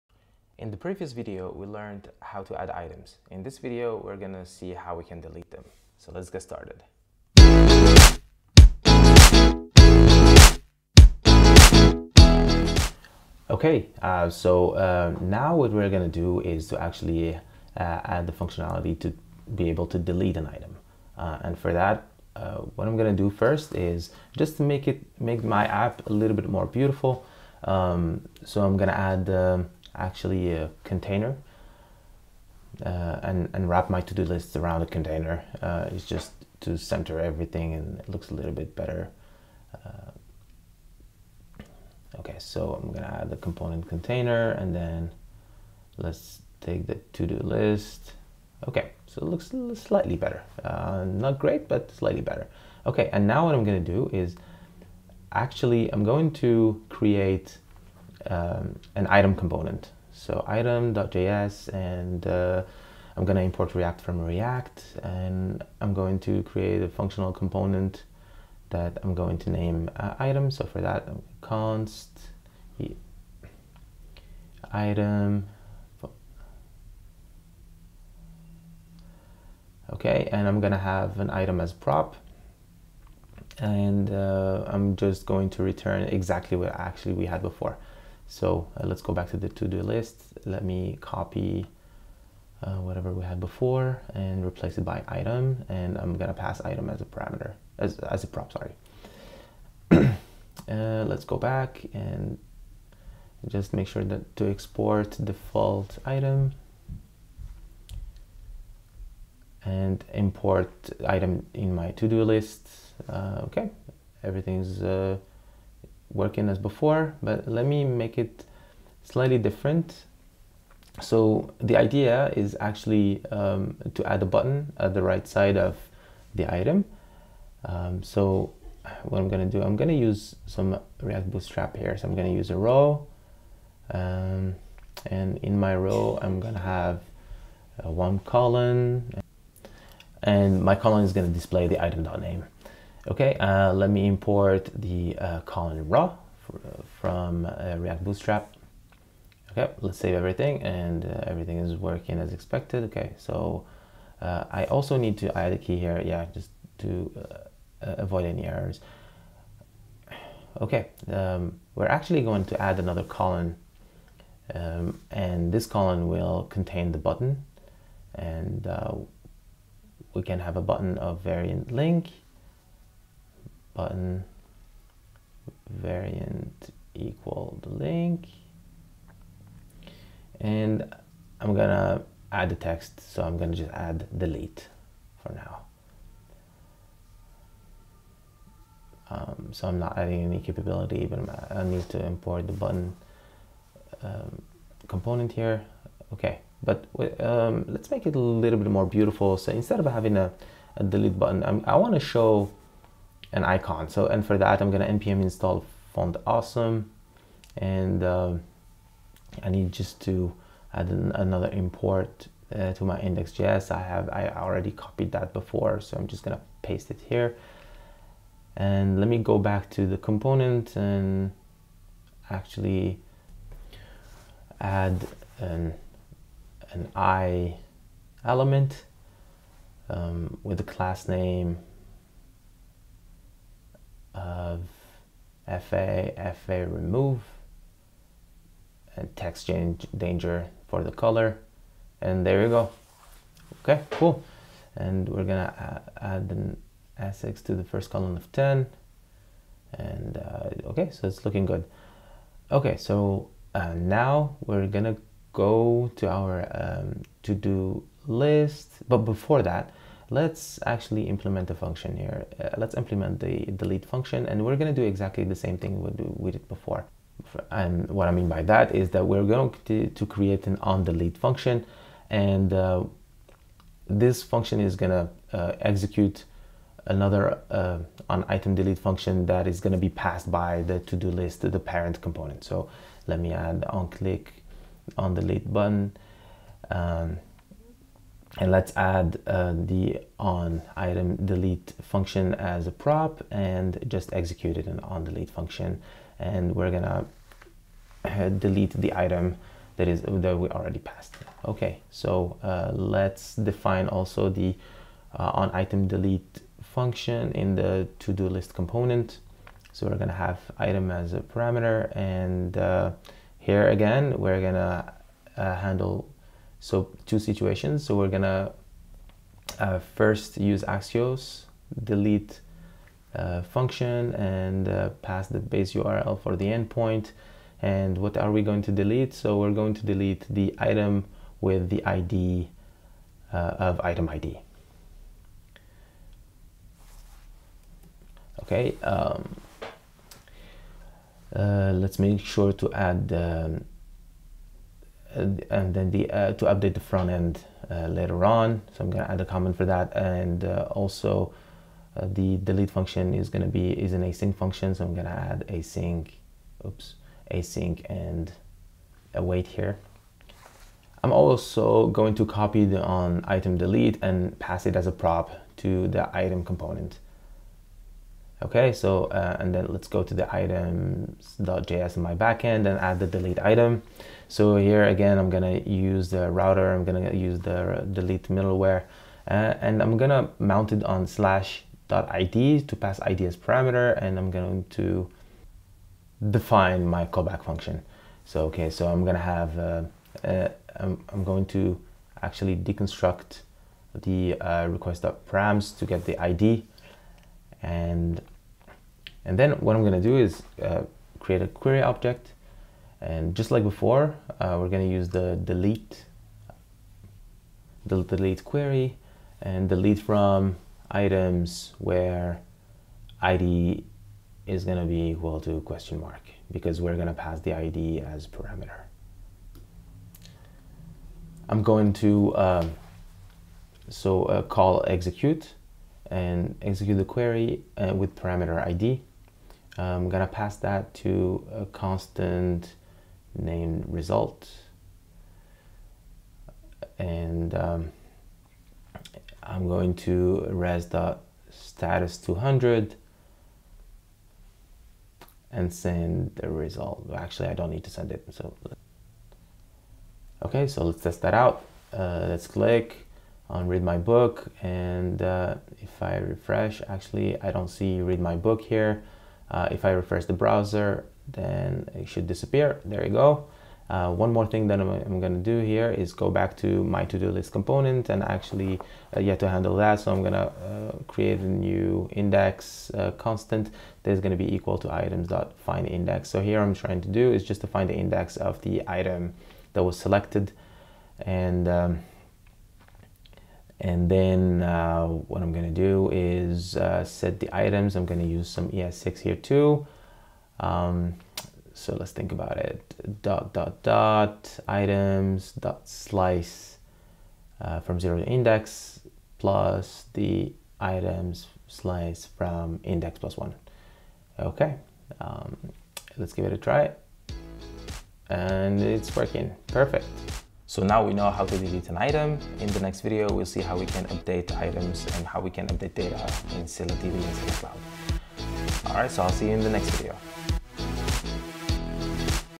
Todo App Part 6: Handle DELETE with ScyllaDB and NodeJS
This video is part of the series to create a Todo app using NodeJS, ReactJS and Scylla Cloud.